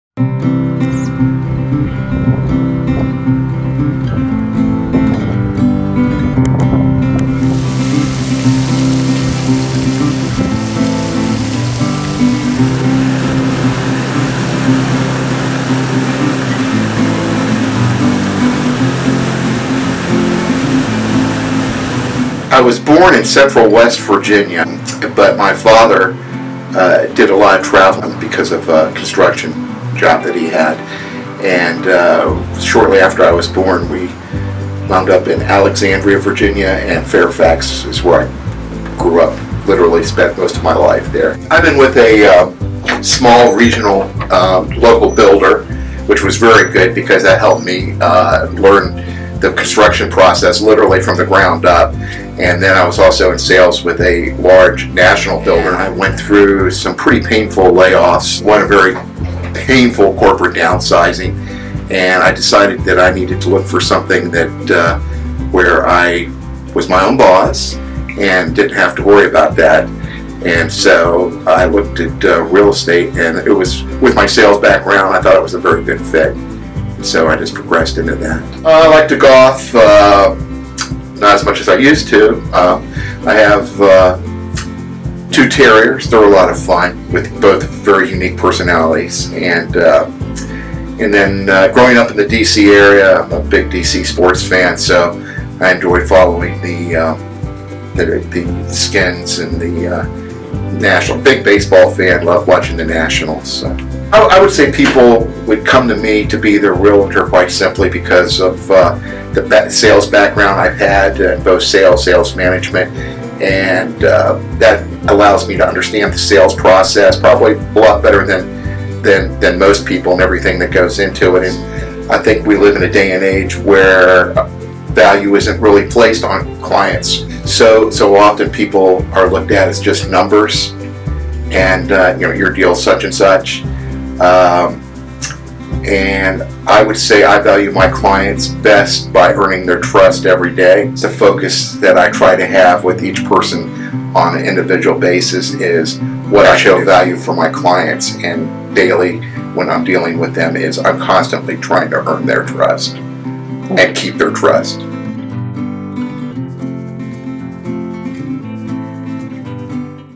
Tim Shamblin, REALTOR™ with Coldwell Banker Premier Properties
Tim Shamblin is a Real Estate agent with Coldwell Banker Premier Properties in Winchester, Virginia. Contact him today!